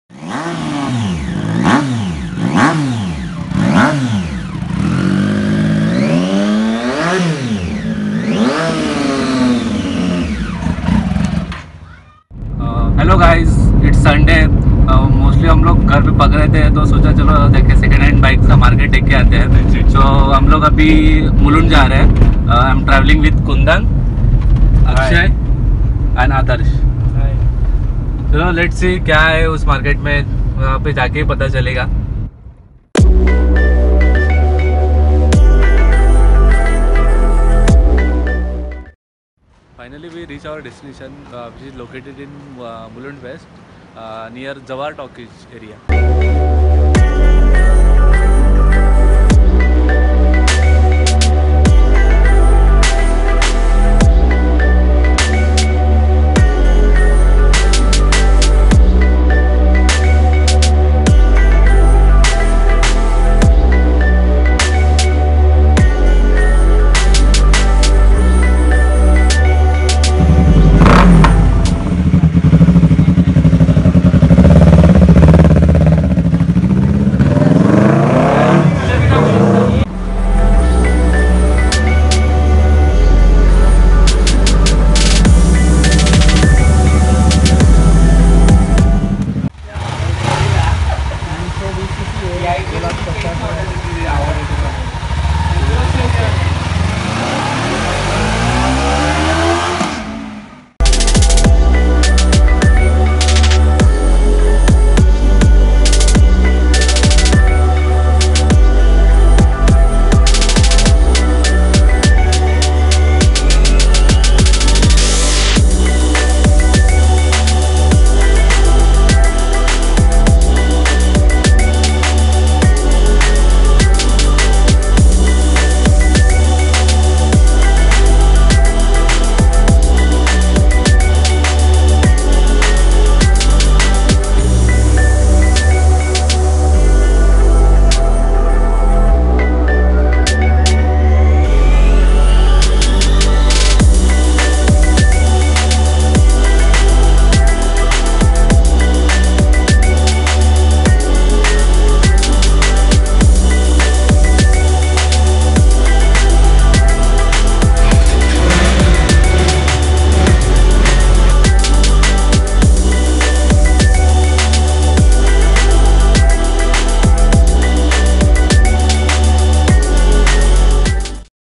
Second Hand Bikes in Mumbai || Latest 2018 || Sport Bikes || Second Hand Bikes Market
Bullet Raja Bike shop which is located in mumbai ( Mulund west )